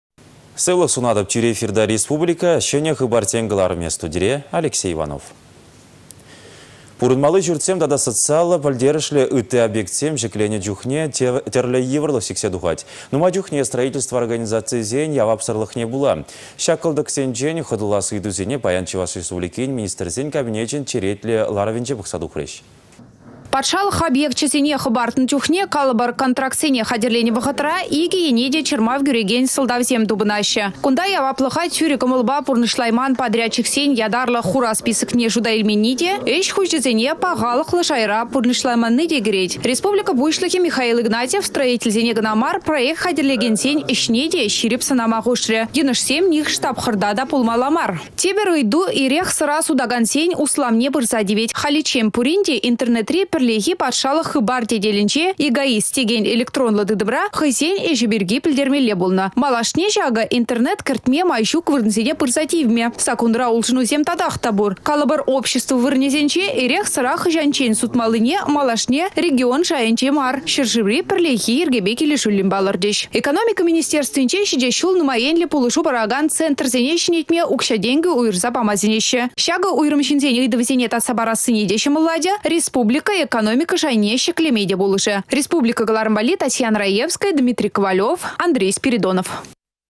Правительство ларăвĕнче
Пурăнмалли çуртсем тата социаллă пĕлтерĕшлĕ ытти объектсем çĕкленĕ чухне тĕрлĕ йывăрлăхсем сиксе тухаççĕ. Нумай чухне строительство организацийĕсен явапсăрлăхне пула. Çак кăлтăксенчен хăтăлас ыйтусене паян Чăваш Республикин Министрсен Кабинечĕн черетлĕ ларăвĕнче пăхса тухрĕç.